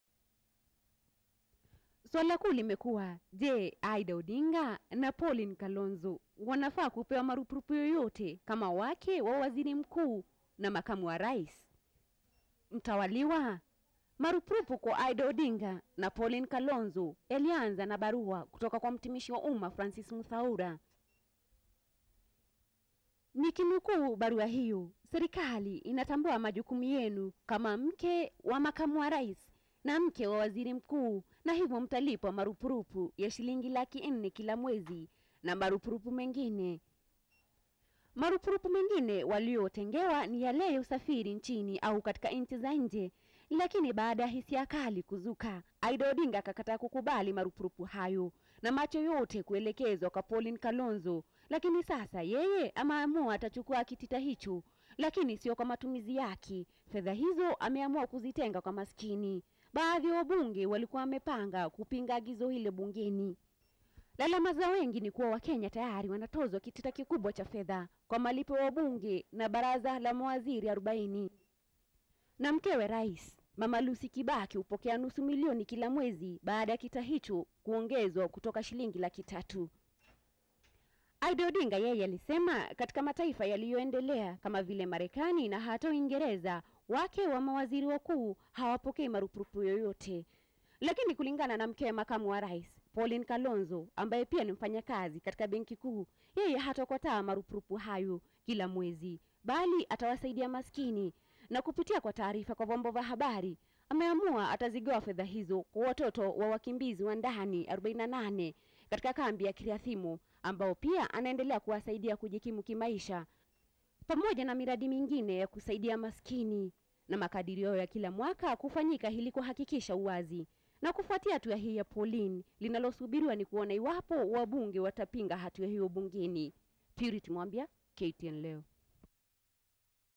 Mkewe Kalonzo achukua marurupurupu
Mkewe Kalonzo Musyoka,Pauline Kalonzo amekubali kupokea marurupurupu ya shillingi laki nne kila mwezi. Lakini Pauline amesema atetenga fedha hizo katika miradi mbali mbali ya kusadia maskini humu nchini. Hatua hii ya Pauline imewadia baada ya marurupu hayo kukashifiwa na wengi na kupelekea Aida Odinga mkewe Waziri Mkuu Raila Odinga kukataa kupokea marupurupu hayo.